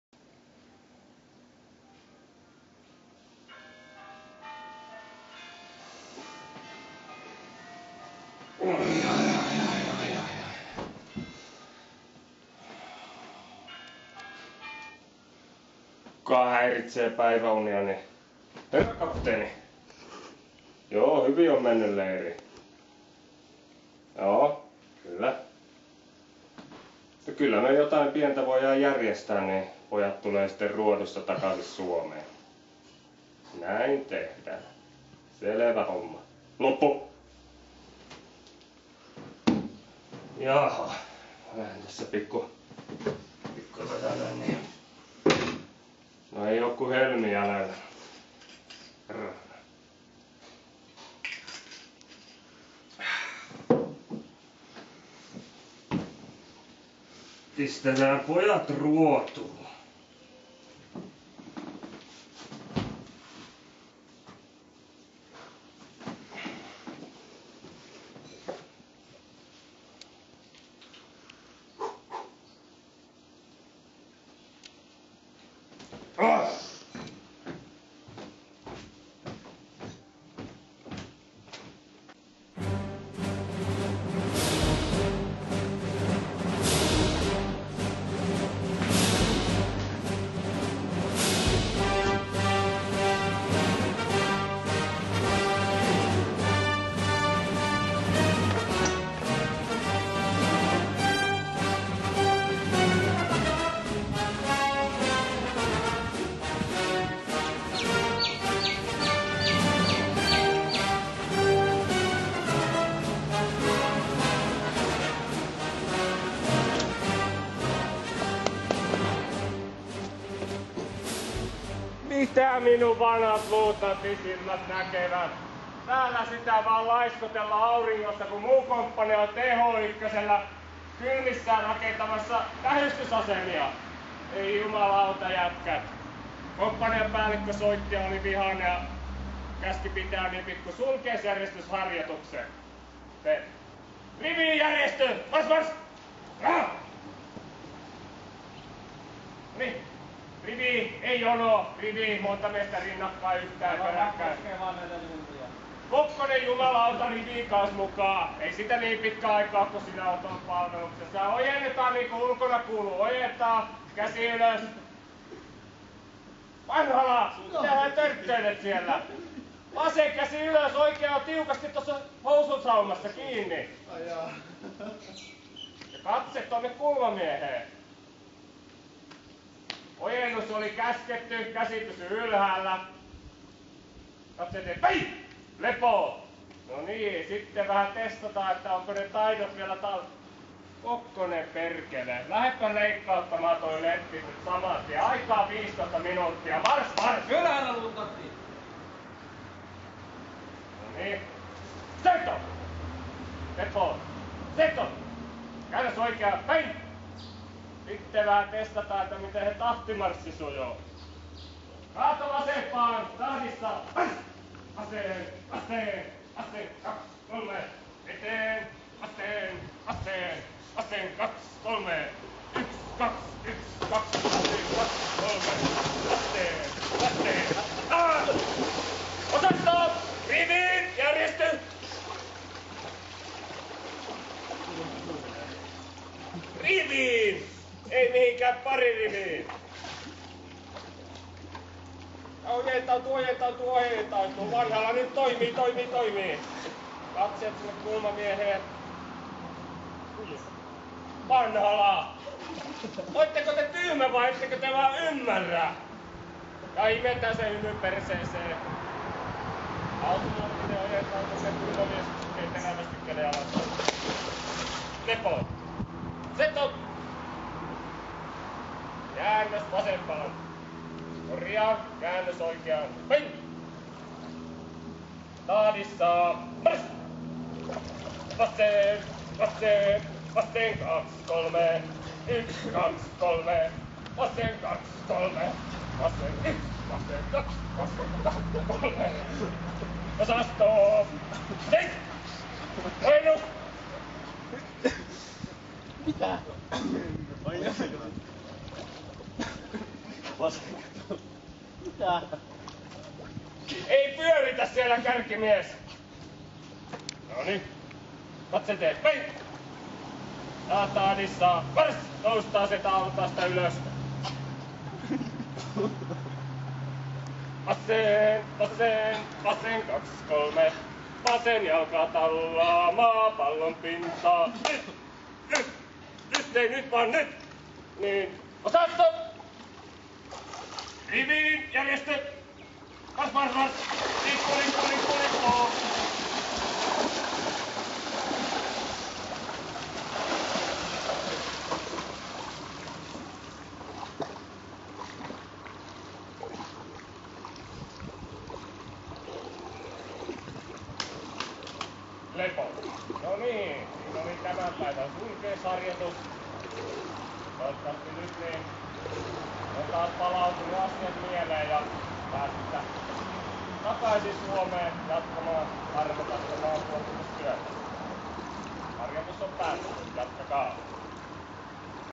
Training camp in Potch (lepopäivä)
Finnish military sport school athletes at training camp in South-Africa